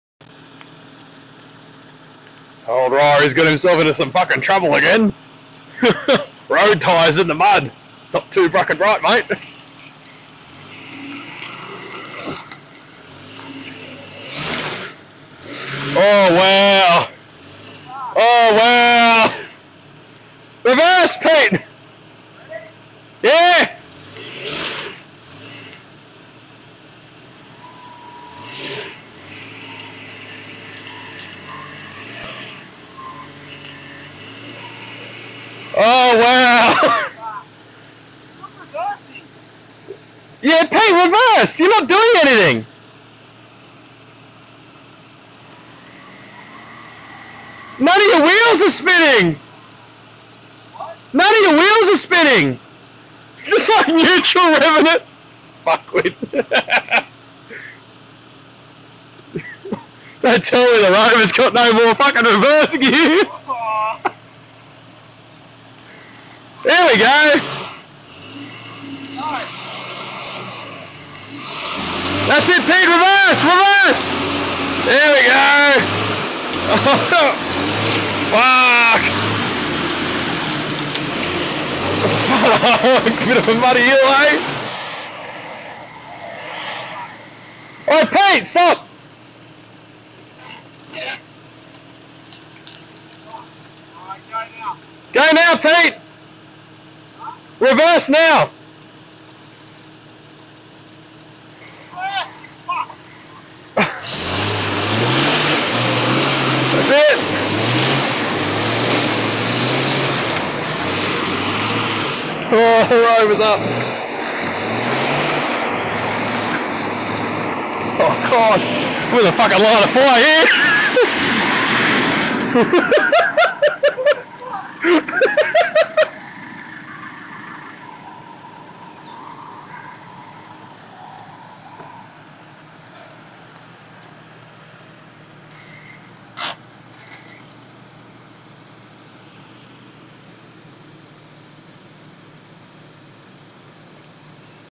4x4 - Brindabella Ranges - Pete Stuck 1
Day trip to Brindabella ranges - Pete looking for reverse